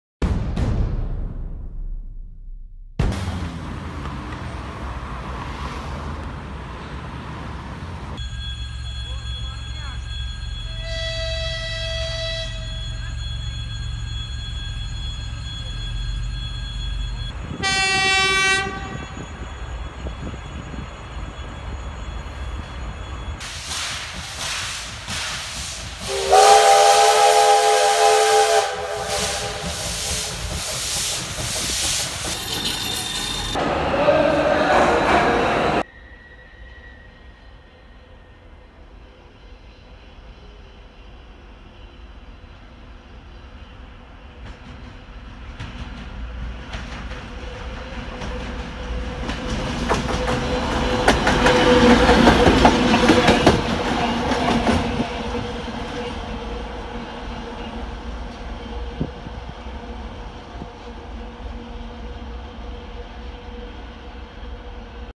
Тепловоз ЧМЭЗт - 7118
Тепловоз Приписан в:
ТЧЭ-12
"Санкт-Петербург-Финляндский"
Следует Резервом
По Перегону
Токсово - Мурино
Участок Пути
платформа Лаврики - платформа Девяткино
2-ой Главный Путь.
Близ платформы "Девяткино".
Финляндское Направление Пути,
Октябрьская Железная/Дорога.РЖД.
Мурино,
Ленинградская Область,Россия.
23 Сентября 2019 Года